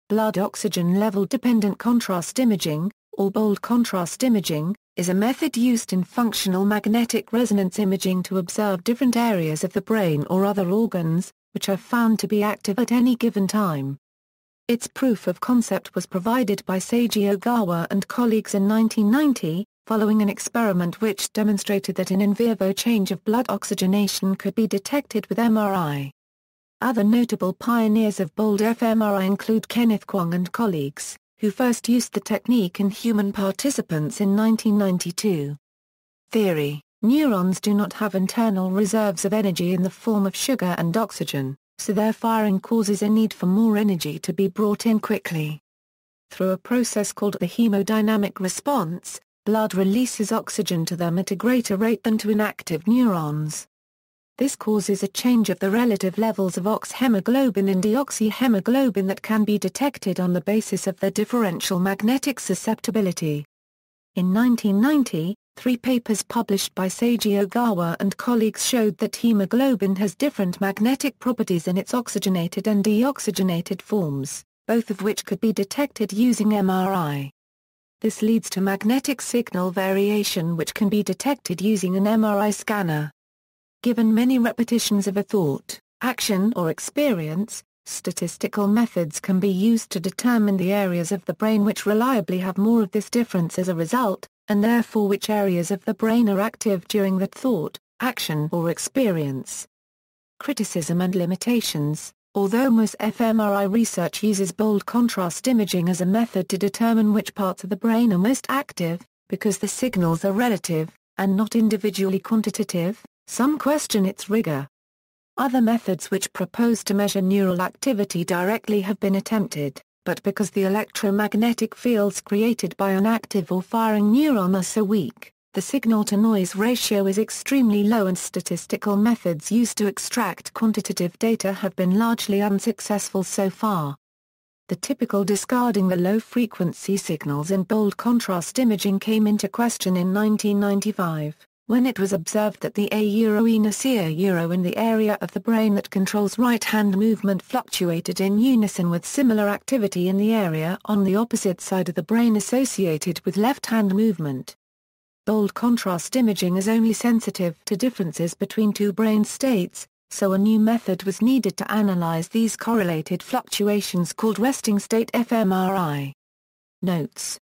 Blood-oxygen-level dependent
Blood-oxygen-level dependent contrast imaging, or BOLD-contrast imaging, is a method used in functional magnetic resonance imaging to observe different areas of the brain or other organs, which are found to be active at any given time. Its proof of concept was provided by Seiji Ogawa and colleagues in 1990, following an experiment which demonstrated that an in vivo change of blood oxygenation could be detected with MRI. Other notable pioneers of BOLD fMRI include Kenneth Kwong and colleagues, who first used the technique in human participants in 1992.